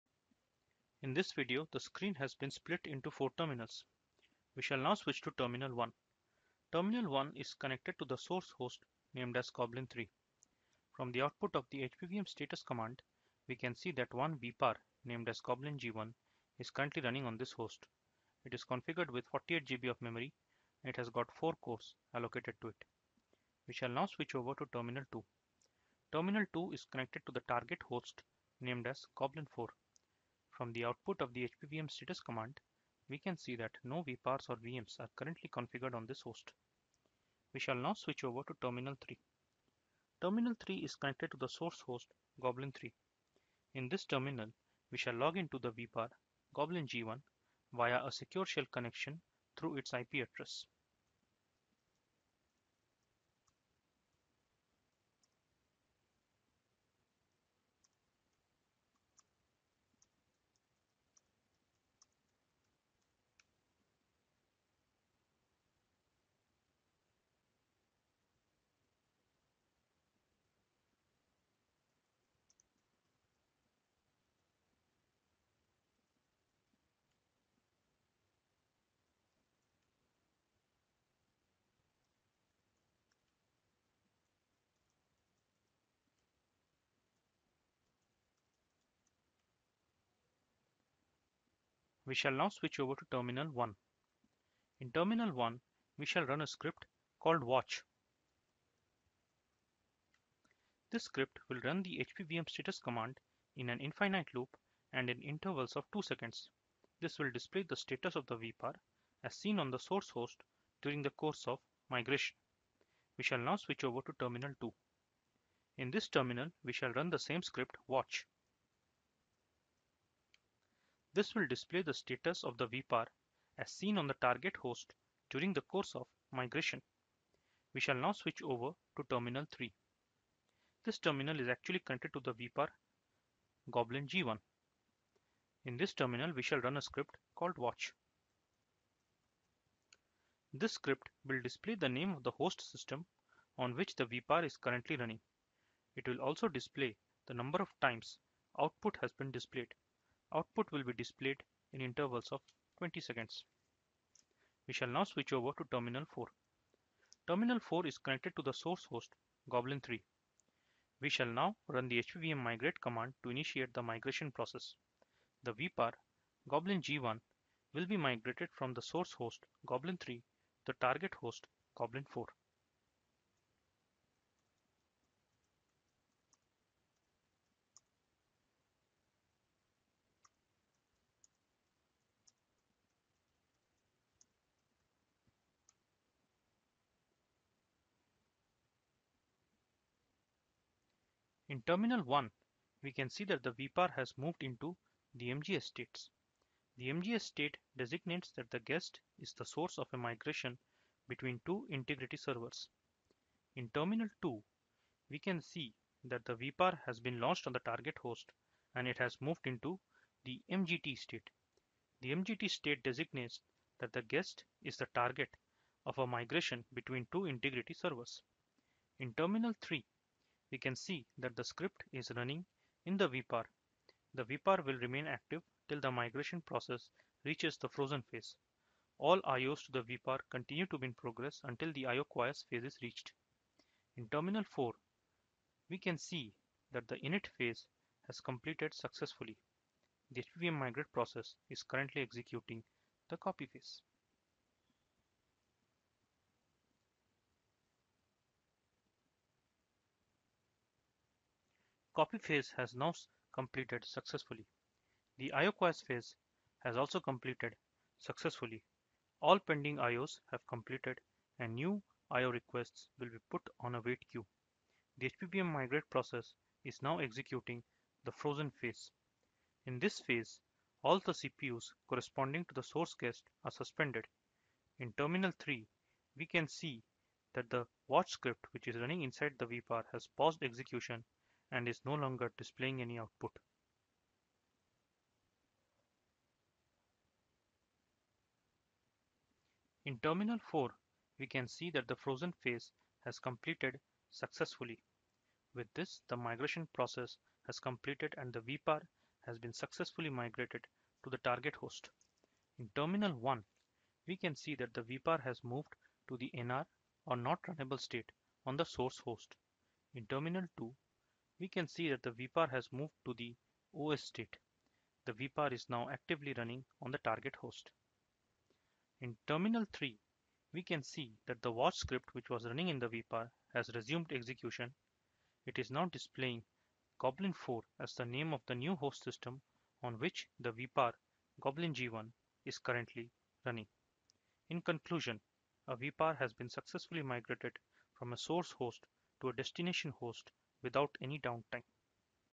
HP-UX How-to: vPar Migration
In this video, we provide a hands on demo for migrating vPar partitions.

Presenter: Shounak Bandopadhyay